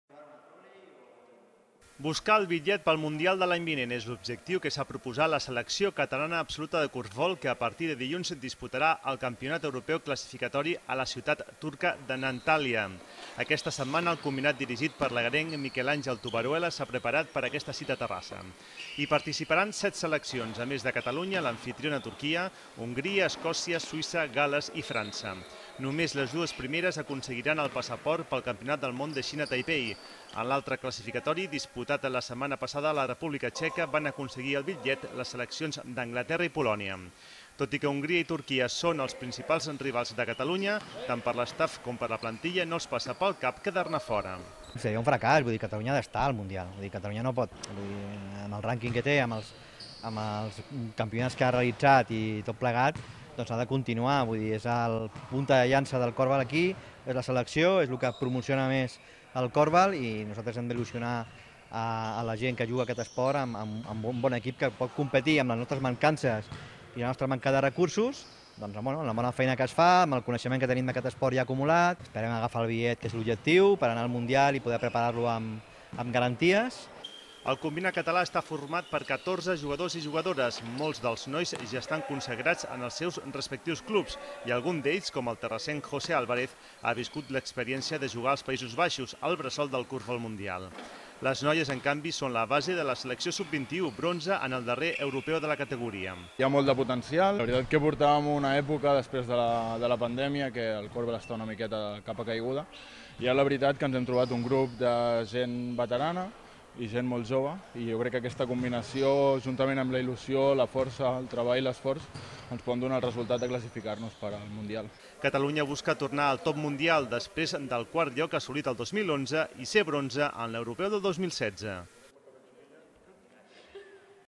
Catalunya busca a Turquia un dels dos bitllets per al Mundial de 2023 de corfbol
El combinat català, dirigit per l'egarenc Miquel Àngel Tobaruela, disputa el classificatori per a la cita mundialista. Més informació a